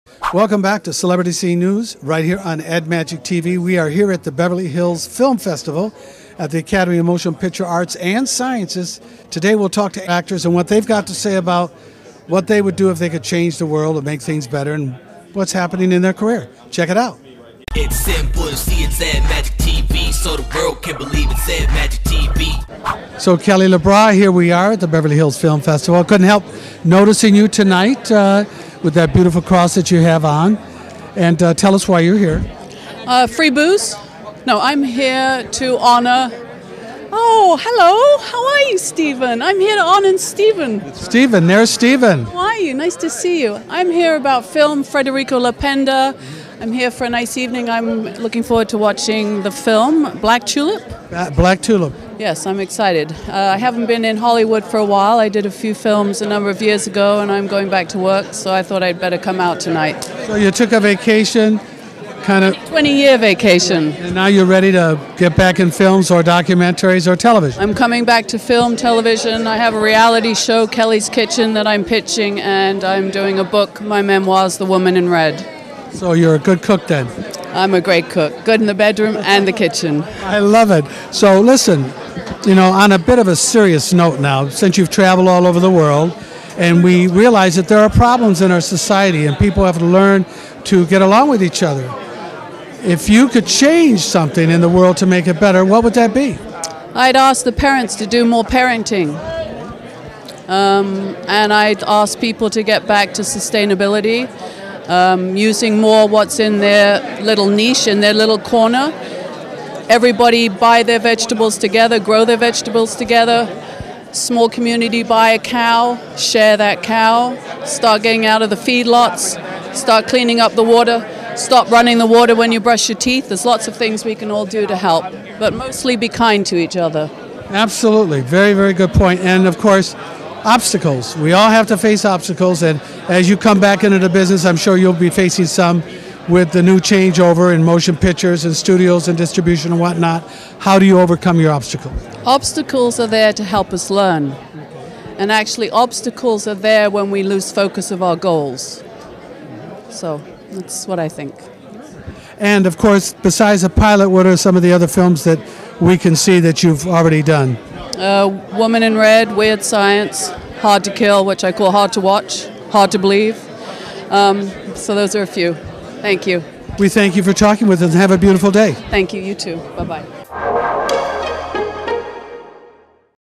Kelly LeBrock Interview at Beverly Hills Film Festival 2012
Celebrity Scene News host Pete Allman interviews actor Kelly LeBrock on the red carpet for the gala opening of the Beverly Hills Film Festival 2012 held inside the Academy of Motion Pictures Arts and Sciences, Beverly Hills, California.